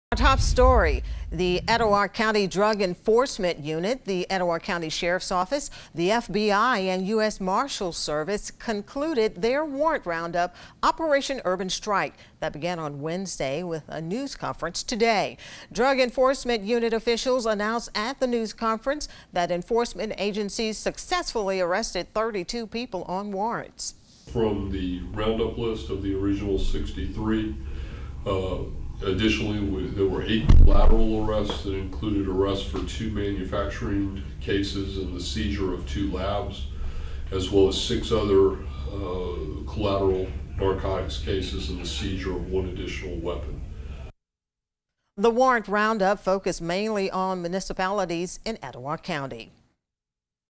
Operation Urban Strike in Gadsden
The Etowah County Drug Enforcement Unit, the Etowah County Sheriff's Office, the F-B-I and U.S. Marshals Service concluded their warrant round-up -- Operation Urban Strike - that began on Wednesday, with a news conference, today. Drug enforcement unit officials announced at the news conference that enforcement agencies successfully arrested 32 people on warrants. The warrant round-up focused mainly on municipalities in Etowah County.